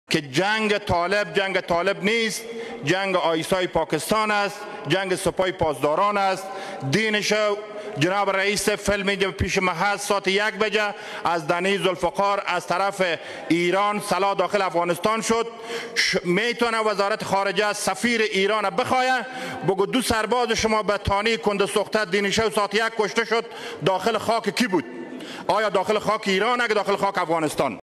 Abdul Sattar Hosseini : The Revolutionary Guards bring weapons to the Taliban.
Abdul Sattar Hosseini, Member of the Afghan Parliament: The Taliban war is in the interests of the Iranian regime. The Revolutionary Guards bring weapons to the Taliban.
Two Iranian soldiers were killed inside Afghanistan.